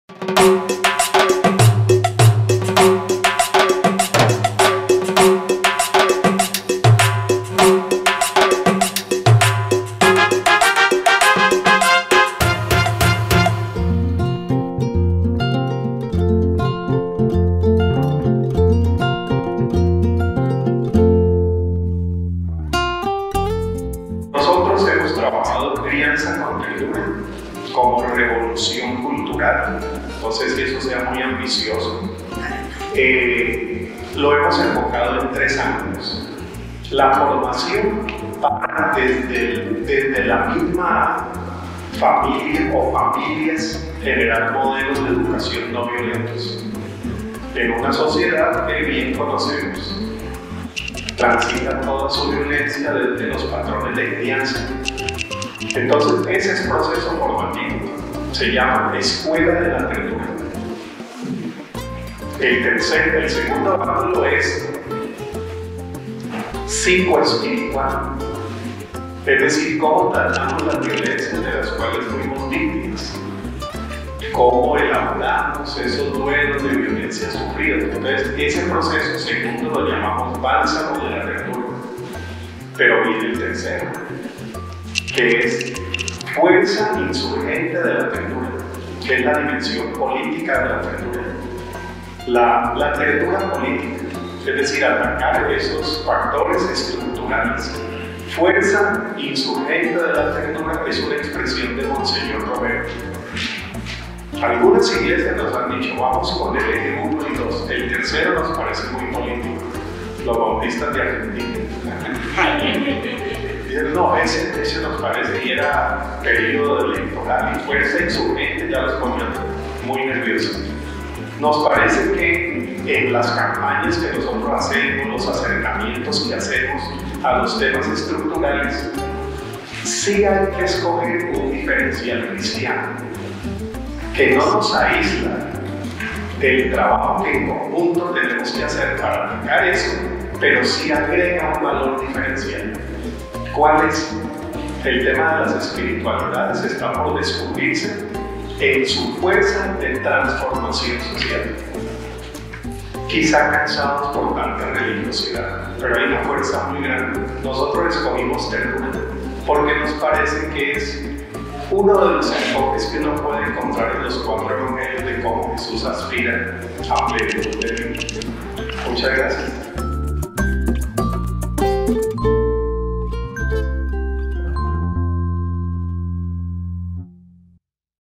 AITR JustPeace Campaign Segura
Hear Harold Segura explain the importance of “parenting with tenderness.” (available in Spanish, English and Portuguese).